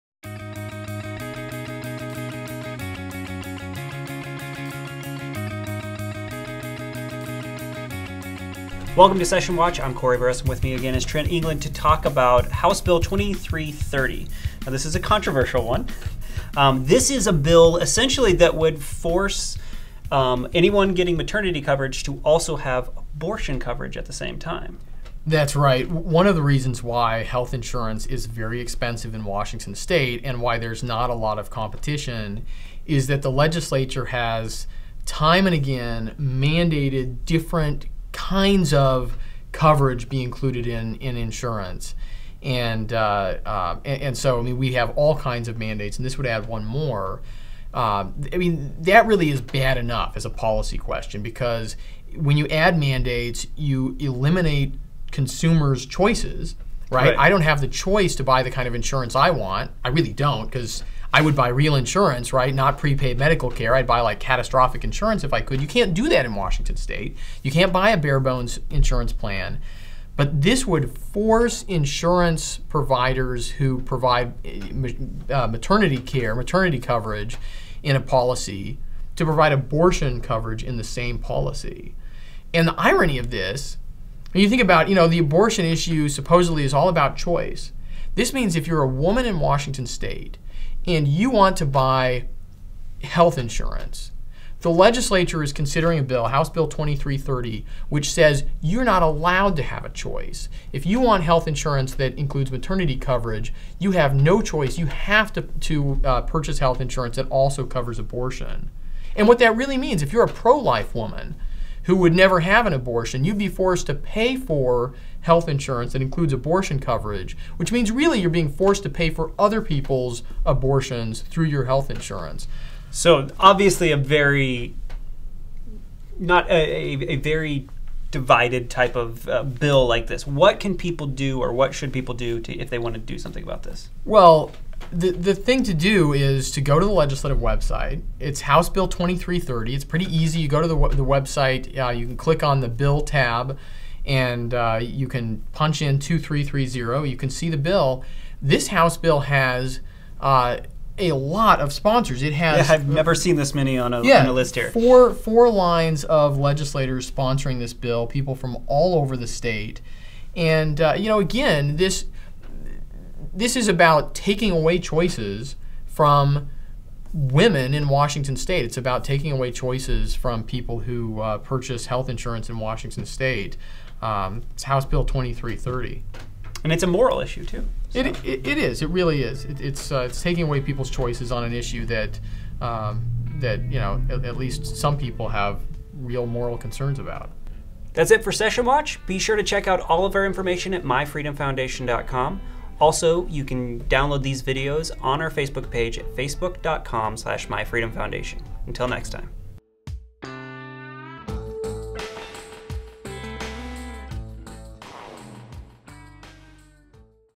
Abortion Coverage - Session Watch
Washington State legislators are considering forcing all insurance in WA to carry abortion coverage with their maternity coverage.  Could this violate Constitutional rights of women in WA?